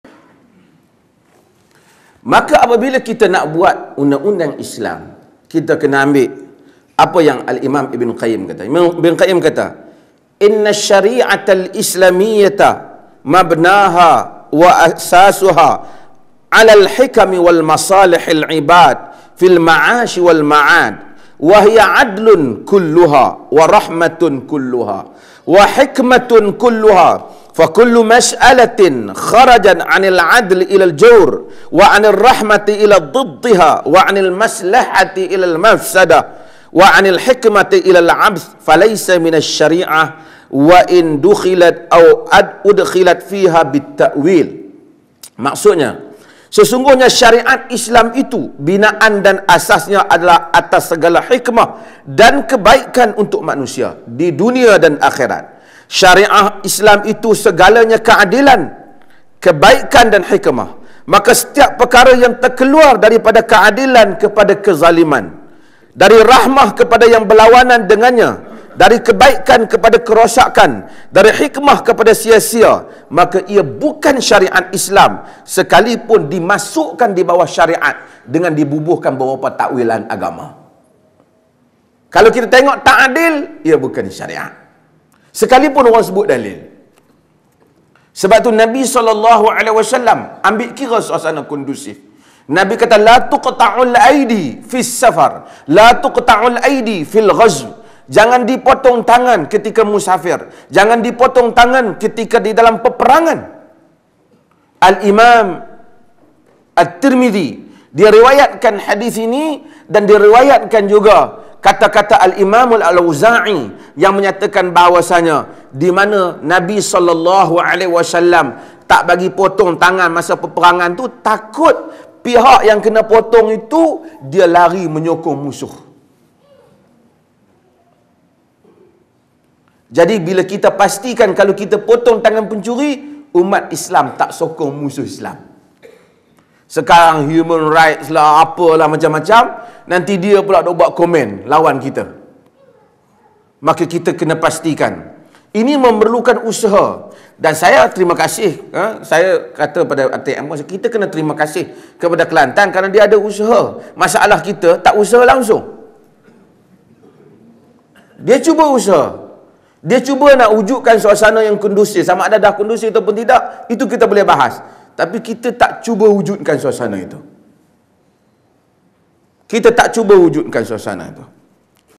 4. Hudud - Perlu Wujudkan Suasana Kondusif - Dr Asri
HD Version - Kuliah Bulanan Prof Madya Dr Asri Zainul Abidin di Lembah Kelang - Riyadhus Solihin - Bab Jihad - Hadith KeTujuh - juga Membawa Isu Semasa iaitu HUDUD. Telah diadakan pada 24 Sep 2011 di Dewan KLSCCCI, Jalan Ampang Kuala Lumpur.